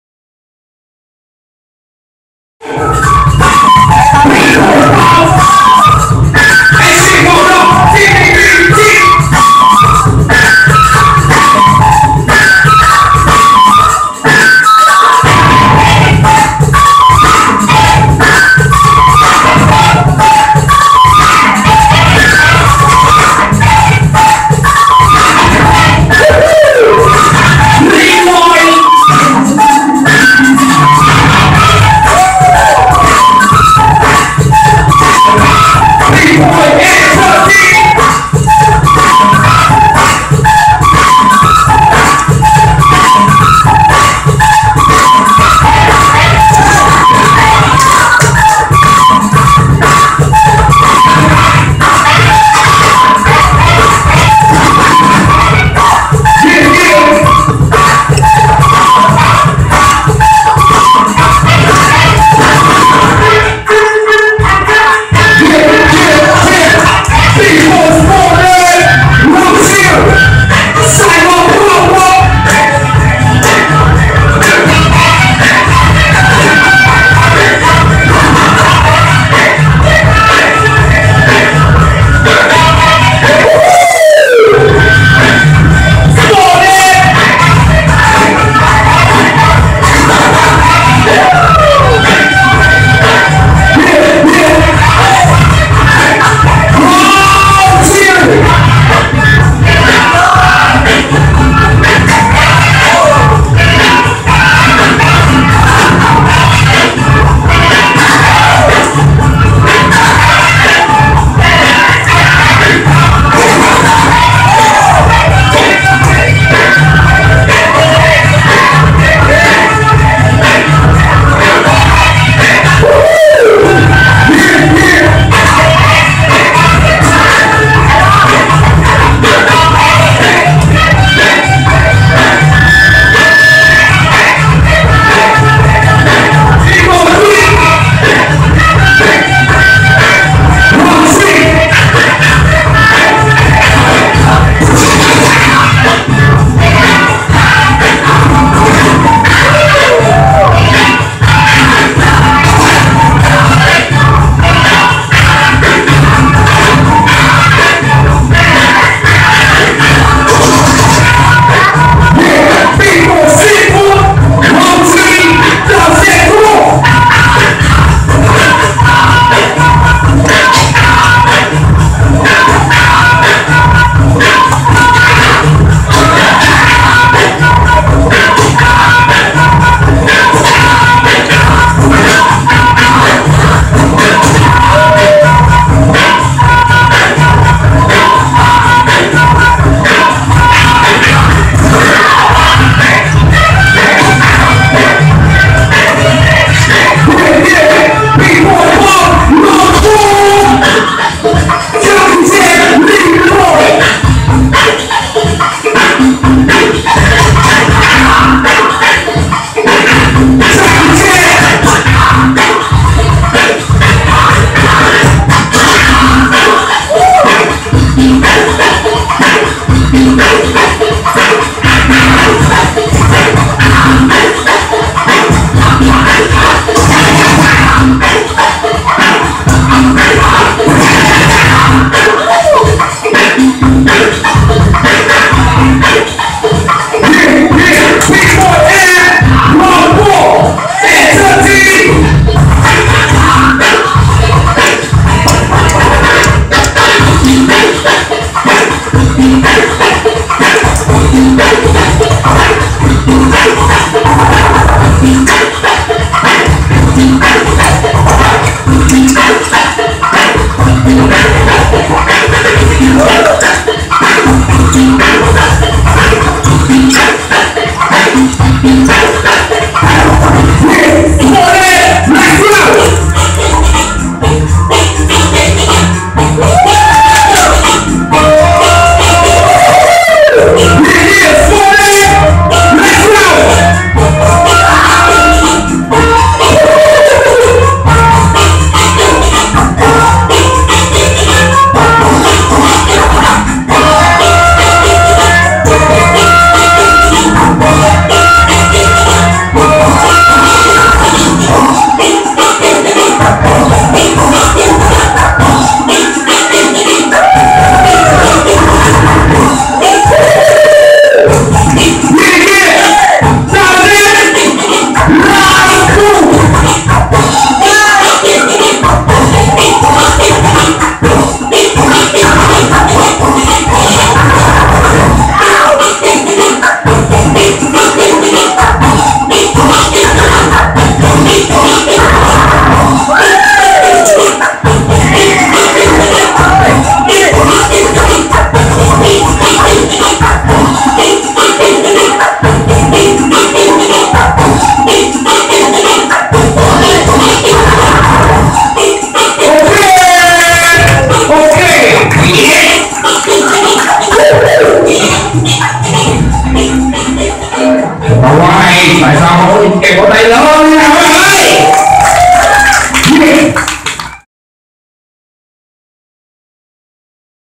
TAOZ vs WILD DOGZ CRAZY | TOP 8 | THE DYNAMIC KINGZ JAM 2015
Winner: TAOZ